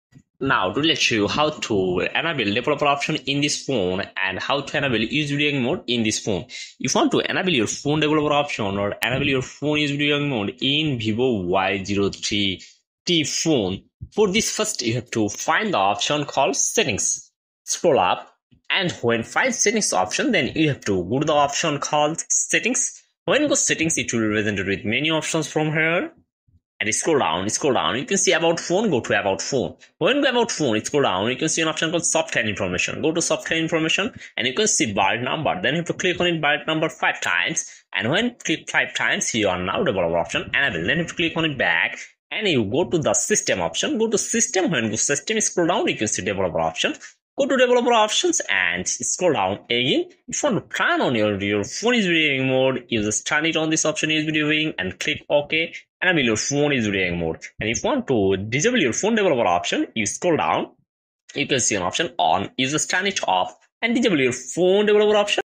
Vivo Y03t Developer option enable // Developer option disable
This phone model is vivo y03t phone. now today i will show you how to enable developer option in vivo y03t phone.

if you want to enable developer  option in this phone.for this first you have to find the option called settings and when find settings go to settings and find about phone go to about phone and find build number option and click three times build number and enable developer option in this phone.

and if you enable usb debugging go to developer option and scroll down scroll down you see usb debugging mode and if you enable usb debugging you just

turn on this option and enable usb debugging in this phone.
and if you disable your phone developer option click on back and see developer option you just turn off this option and disable your phone developer option in vivo y03t phone.